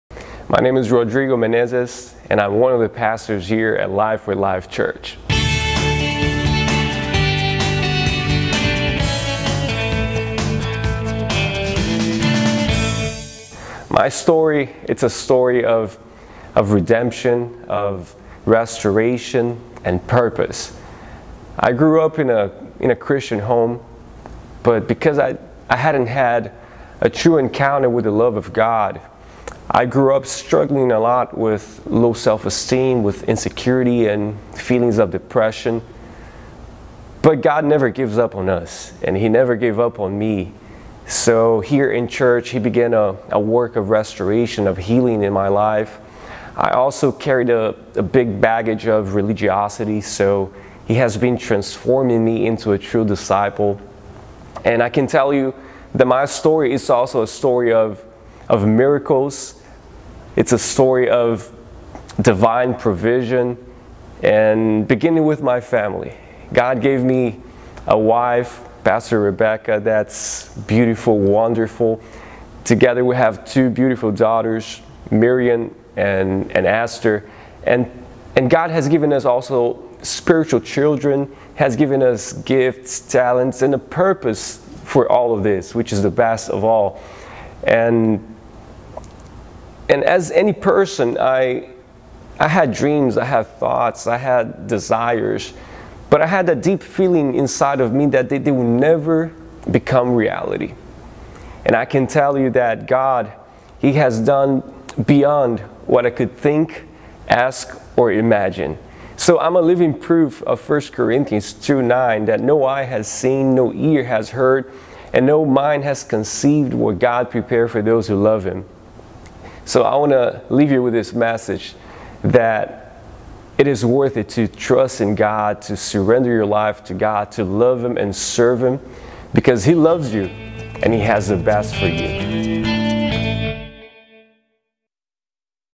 Pr Rodrigo Menezes - Meet the Pastors
Meet Pastor Rodrigo Menezes, one of the pastors here at Life with Life Church